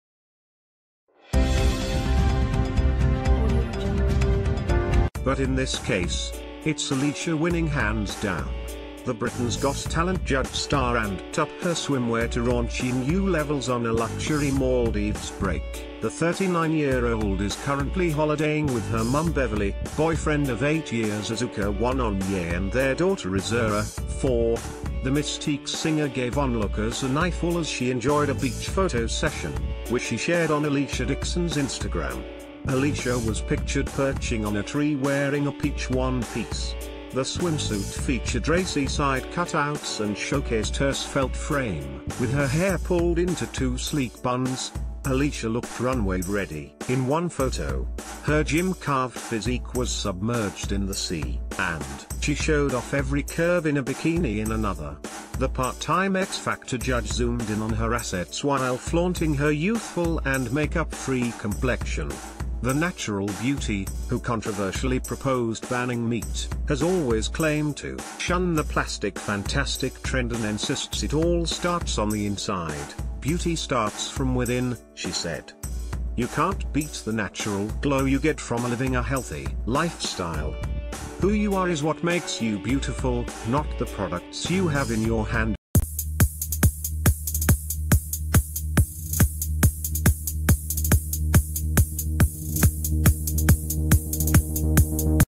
Alesha Dixon reveals every curve in very skimpy swimwear
But in this case, it's Alesha winning hands down. The Britain's Got Talent judge star ramped up her swimwear to raunchy new levels on a luxury Maldives break. The 39-year-old is currently holidaying w...